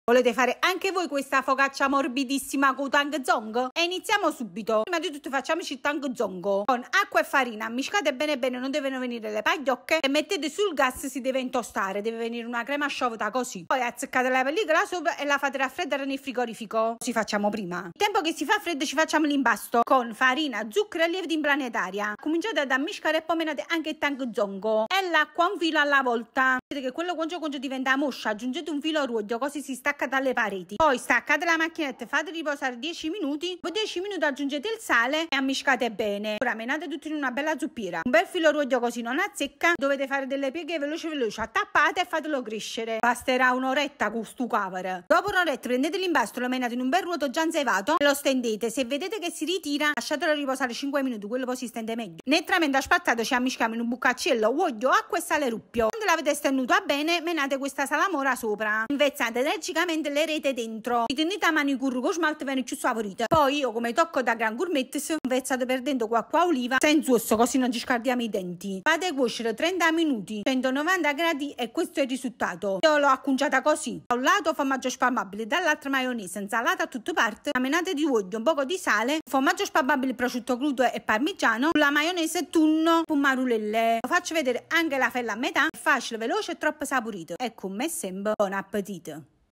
Focaccia morbidissima qui⬅️
Focaccia morbidissima,
Ingredienti ⤵️
✅Tang zhong con acqua,
100 ml di acqua,
20 g manitoba,
✅Impasto
360g di manitoba,
(Aggiungete in tang zong)
10g di lievito,
15g di zucchero,
220ml di acqua,
8g di sale,
✅salamoia
2 cucchiai di olio extravergine,
4 cucchiai di acqua,
1 cucchiaino di sale doppio,
✅ripieno
Philadelphia,insalata,
Crudo,scaglie,
Maionese,insalata,tonno,pomodorini,
Sale,pepe,olio ex,